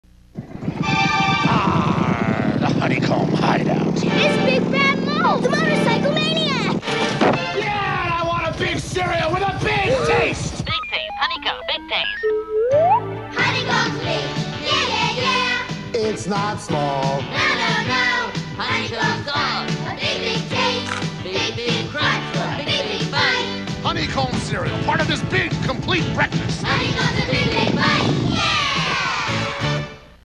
Honeycomb ad, 1988
Classic late '80s spot for the Post cereal that has "big taste".